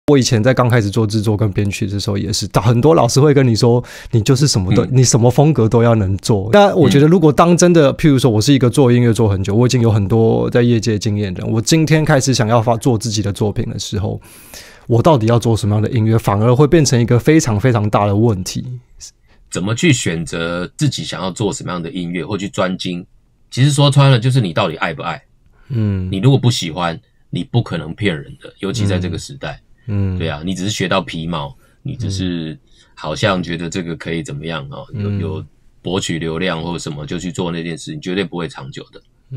我所有風格都會做但我要做哪一種？(feat. 前北流執行長 呂聖斐)
全部內容：獨立音樂島Podcast EP403 || 前北流執行長：補助根本就應該廢除 (feat. 三金音樂總監 呂聖斐)